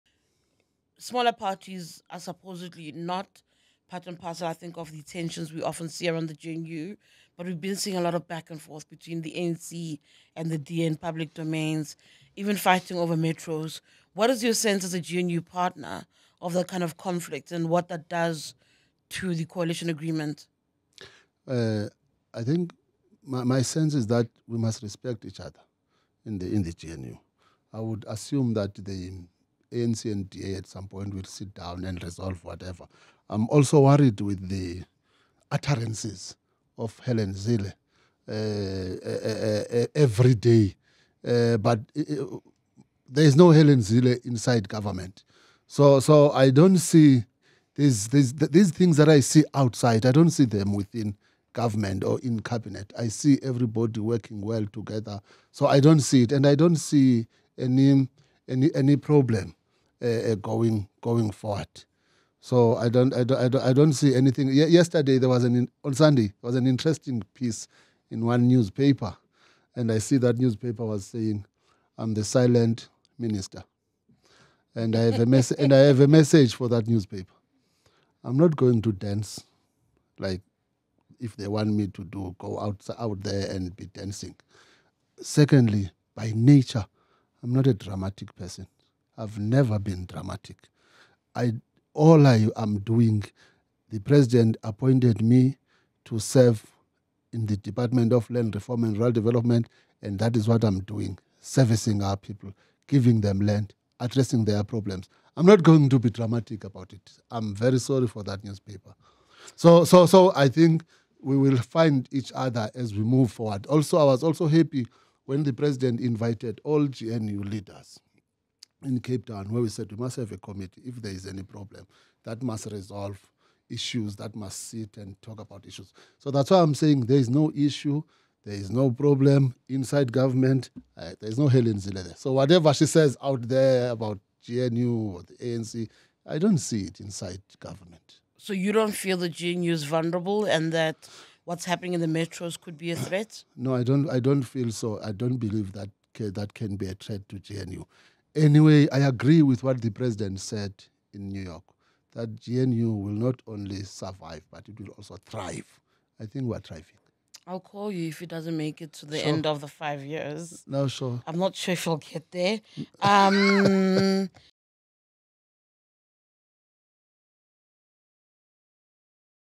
PAC President Mzwanele Nyhontso on GNU: everyone is working well together
EWN Associate Politics Editor Tshidi Madia sat down Rural Development and Land Reform Minister, and Pan Africanist Congress’ (PAC) president Mzwanele Nyhontso to talk about the Government of National Unity and more.